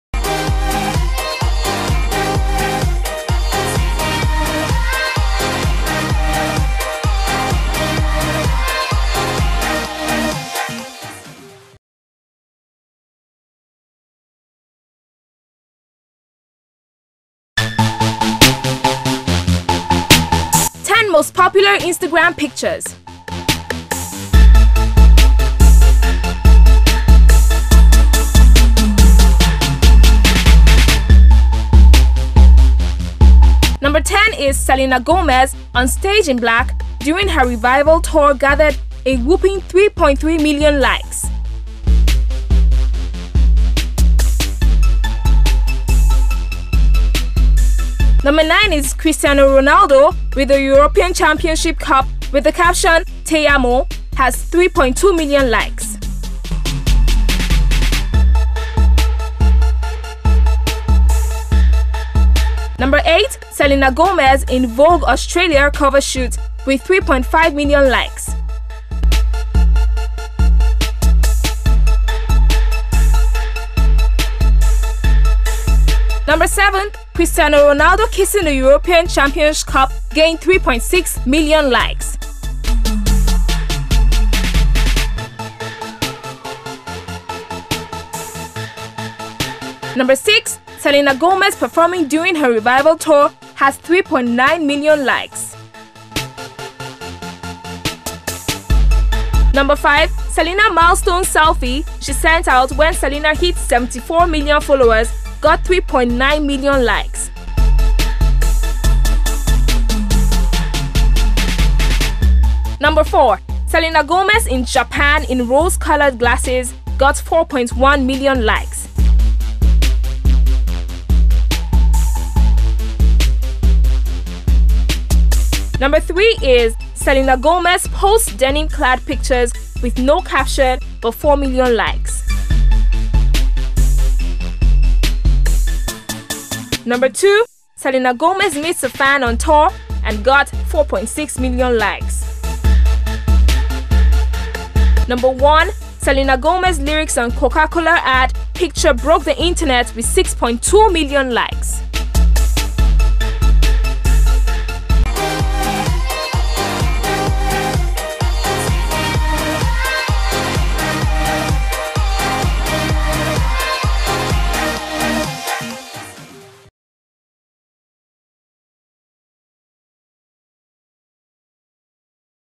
Checkout Ten Most Popular Instagram Pictures Of Celebrities
Yippe! You will agree with me guys, that the most liked photos of celebrities on instagram really shows how much we love them (our celebs).

For more deets.
Watch the video and do enjoy your view.